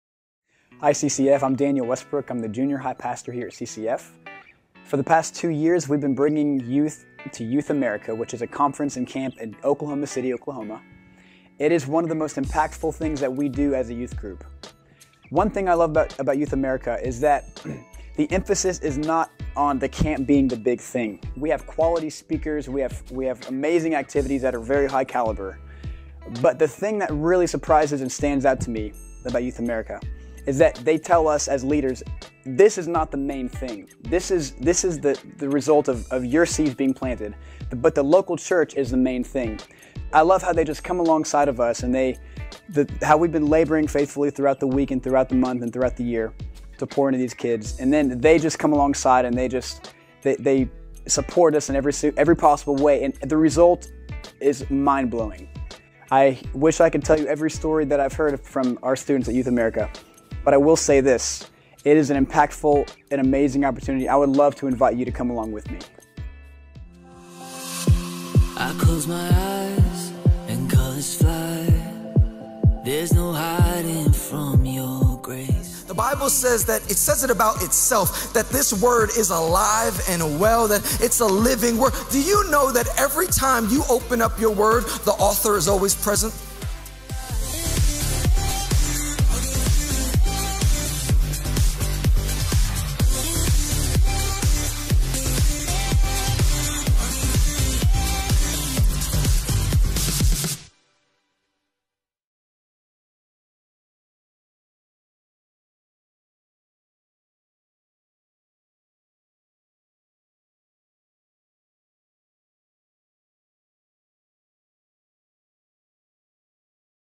Daniel Westbrook: Youth America Promo
CCF's Jr. High School Youth Pastor, Daniel Westbrook, tells about the upcoming Youth America summer camp the youth will be attending June 3-7, 2019. Youth, grades 6-12 ("Ledge78" & "Edge" groups) are welcome to participate.

Register early to get the $219 rate! A $50 deposit is due Feb. 28th.